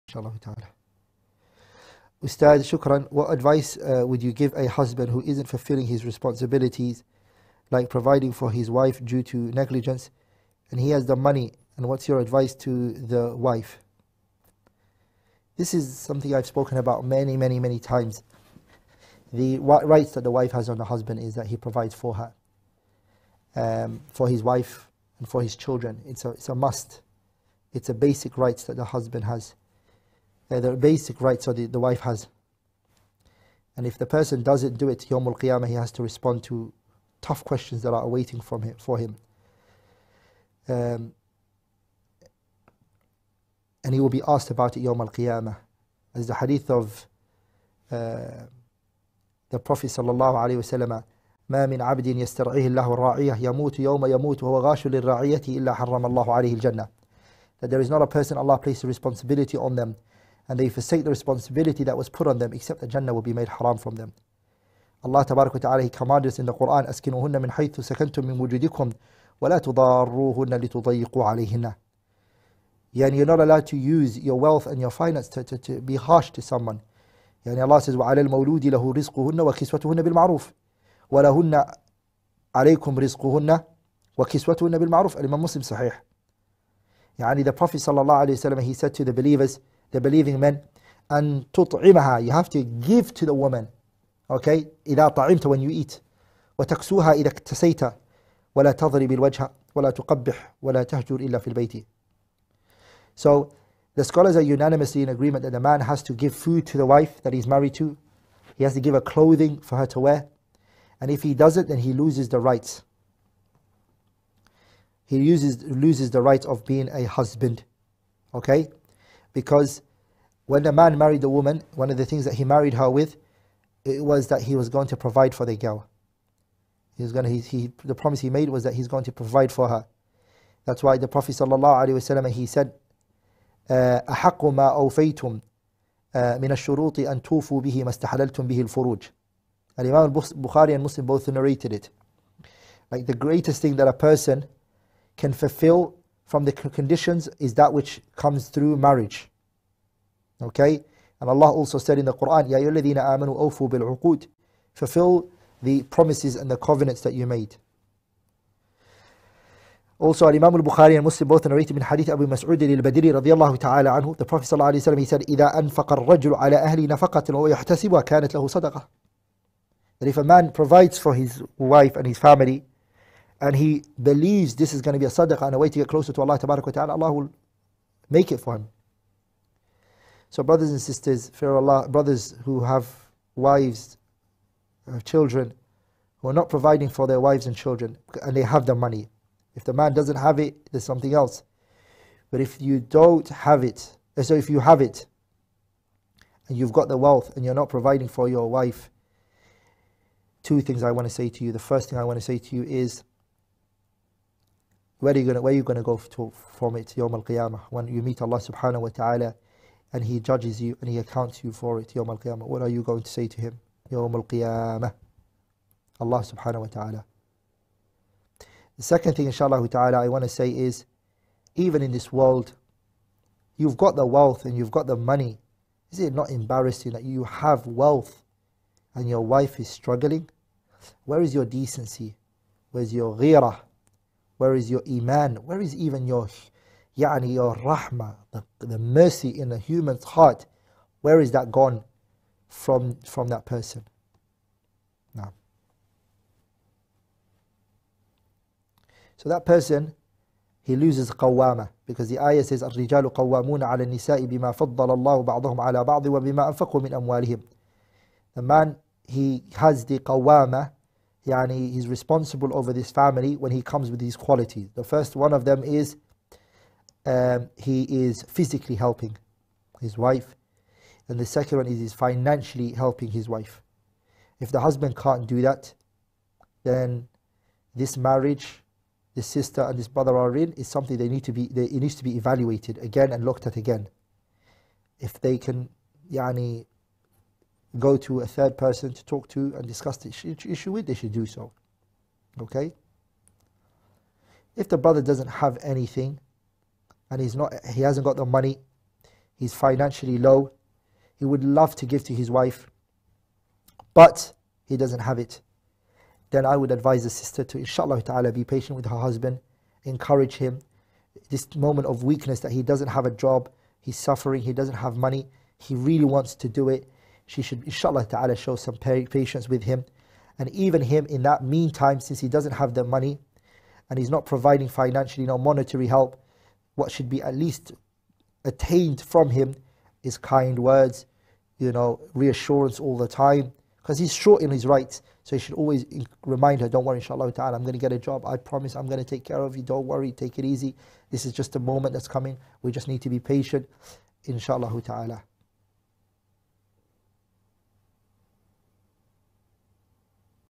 What Advice Would You Give a Husband Who isn’t Fulfilling his Responsibilities
what advice would you give a husband who isn’t fulfilling his responsibilities like providing for his wife due too negligence and he has the money, and what’s your advise to the Ustadh AbdulRahman